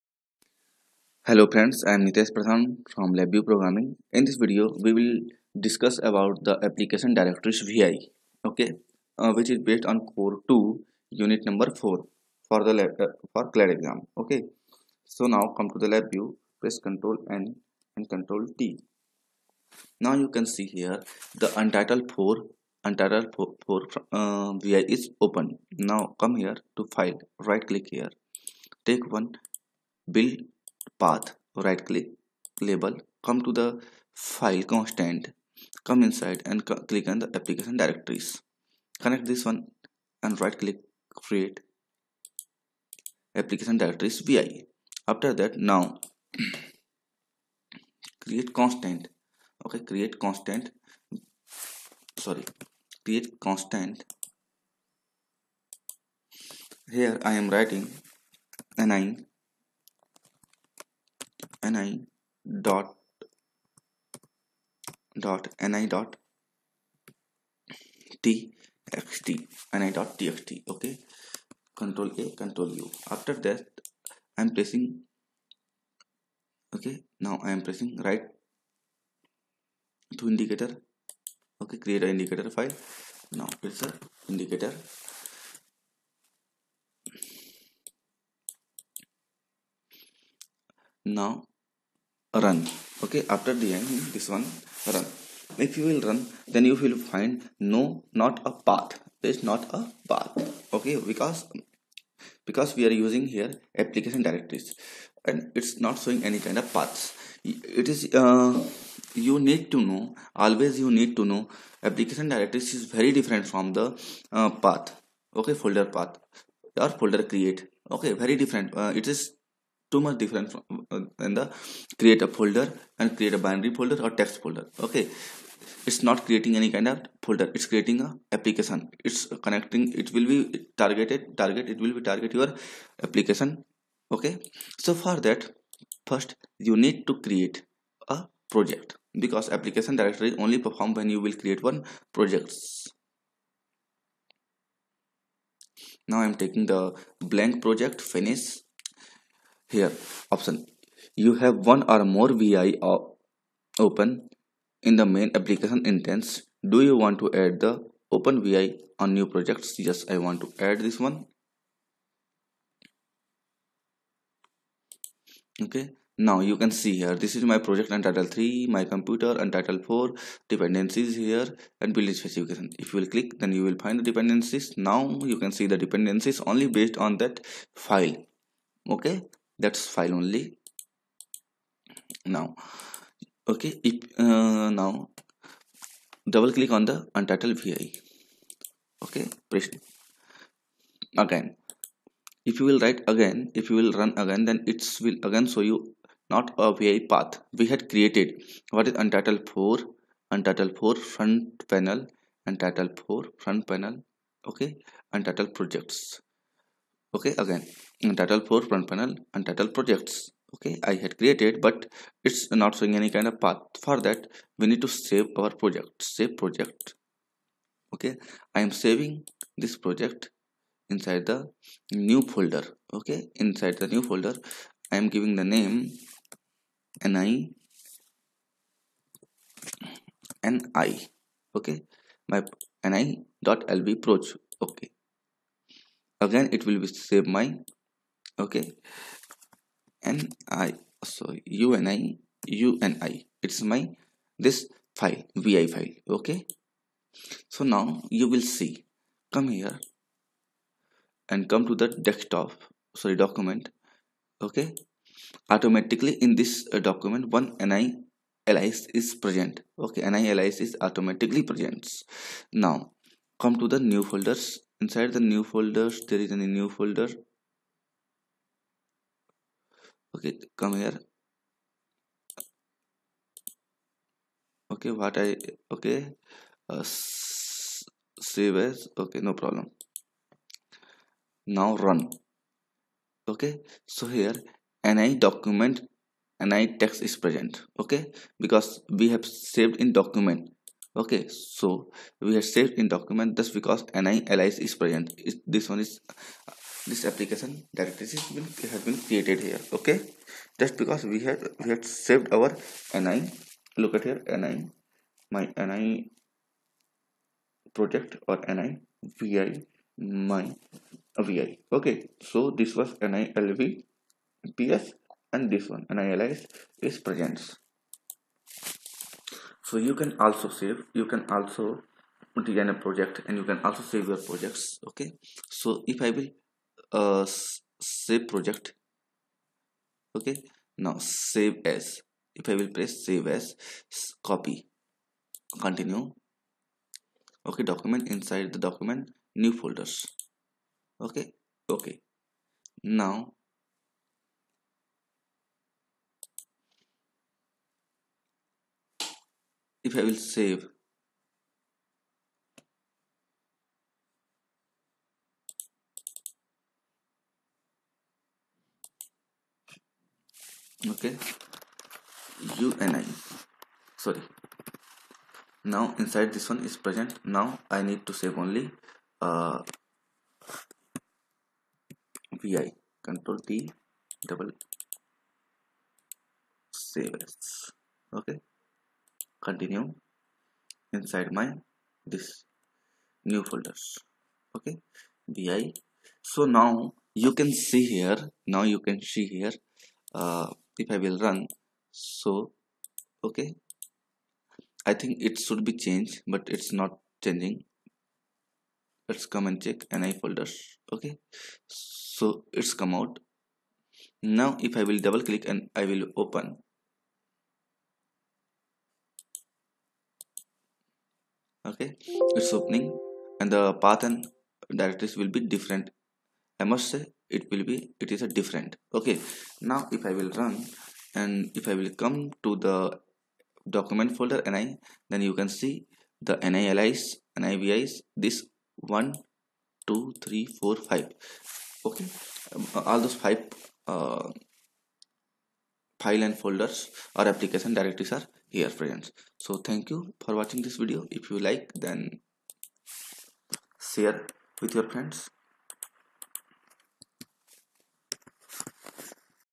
Application directory | How to Create| LabIVEW Core 2 | Nitesh Pradhan
Returns the path to the directory containing the application.

If you call this VI from the development environment and the VI is loaded in a LabVIEW project file (.lvproj), this VI returns the path to the folder containing the project file. If the project is not saved, this VI returns Not a Path.

If the top-level VI is not loaded in a project, the VI returns the path to the directory containing the top-level VI. If the VI is not saved to disk, this VI returns Not a Path.